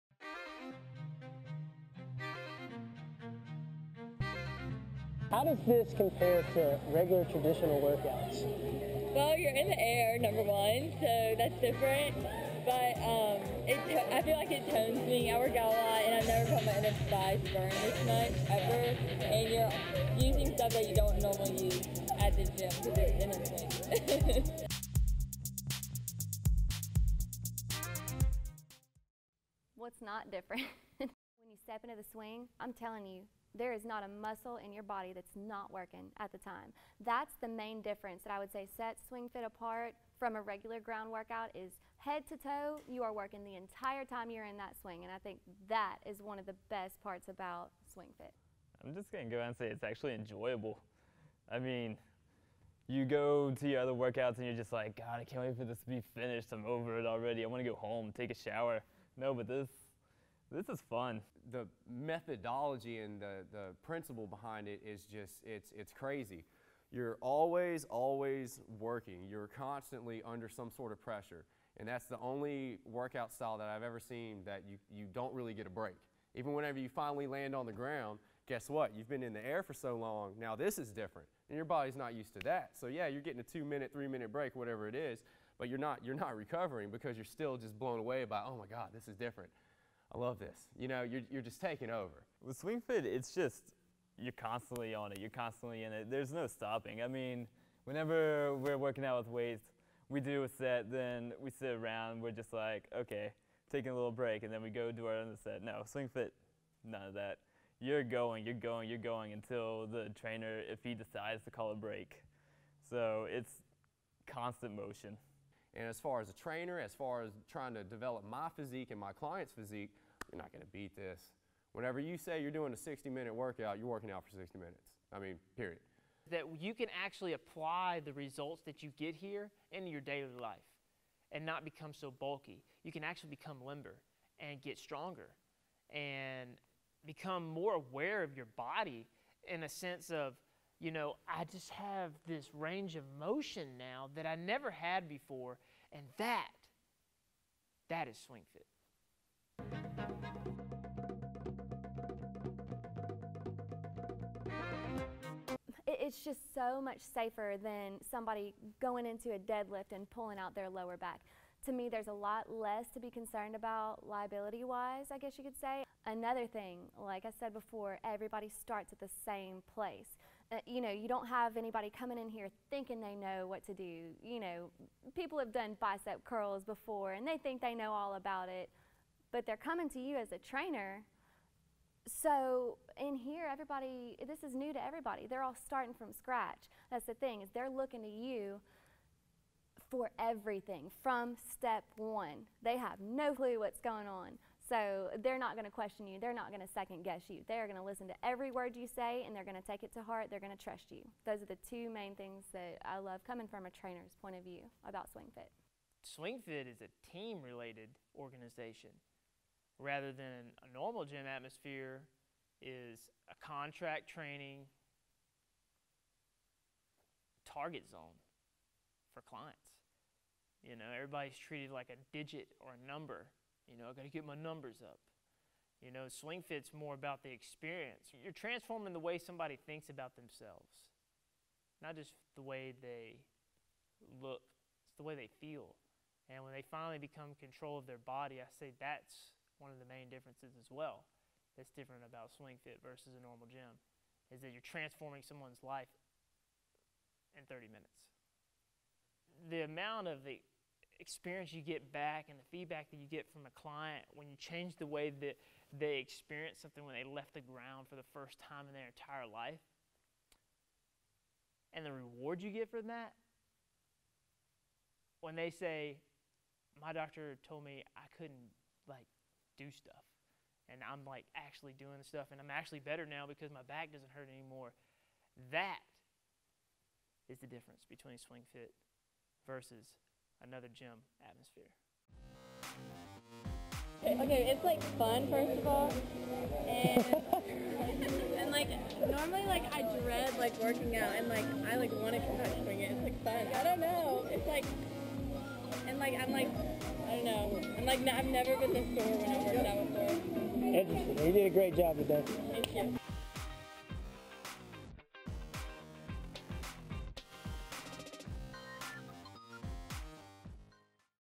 What makes SwingFit so Different
This segment covers, "What makes SwingFit so Different", & "Difference between Training at SwingFit vs Your gym Atmosphere".